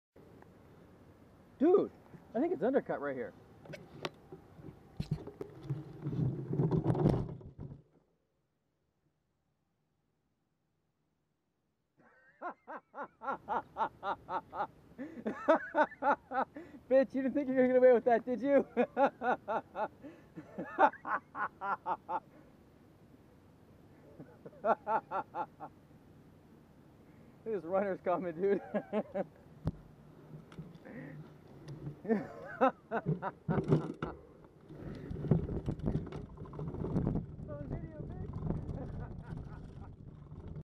Peeing at the edge of America HD
I caught my buddy peeing off a cliff into the Pacific Ocean.